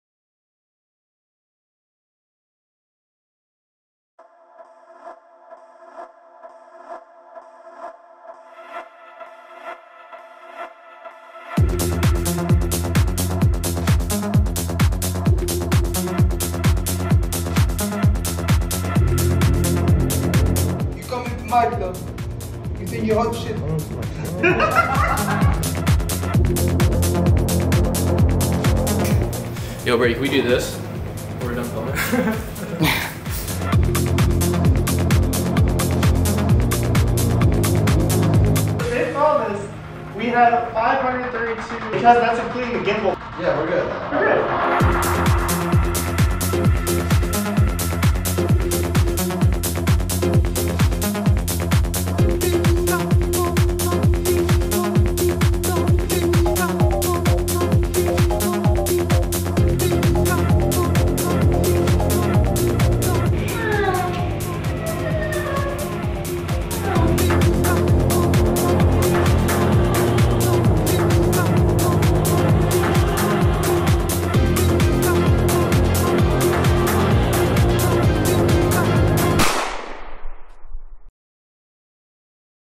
Night Wire Behind The Scenes | Coming Soon
A short film based on Hotline Miami. The film follows Mask, a ruthless killer who carries out hits from a man on the telephone. However, everything isn't as it seems.

Coming soon.

Thanks to all of our backers on Kickstarter!

Keep up to date by following our Instagram @pipehousetv